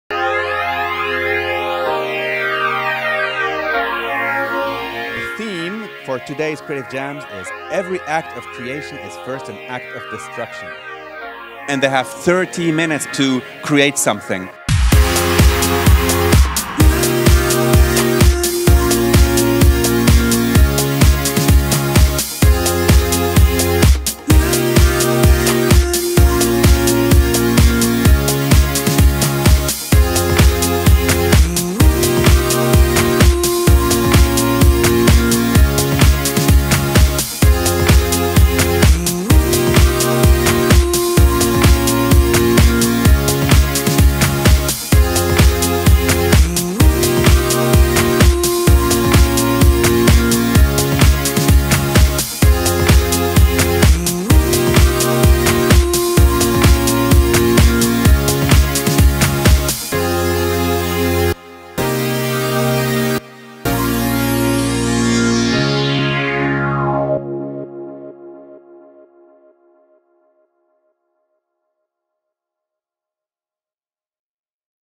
Adobe Creative Jam Devine Howest - Bump Festival Kortrijk
Watch the recap of the first Adobe Creative Jam in Kortrijk. Lotte Bijlsma, Thibault Maekelbergh and Julie van Beethoven are facing Jonas Devacht, Kevin Hendrickx and Bernd Bousard in a 30 minute creative battle.

Bump Festival was all about creativity. Imagine amazing Creative Jam’s and inspiring quotes from top designers and agency’s. More than 600 multimedia developers, designers, entrepreneurs, multimedia students and educators gathered at BUMP to share knowledge, get inspired and network in Kortrijk on Friday the 24th of June 2016. Watch the video and be inspired by the Creative Jam of these young potentials.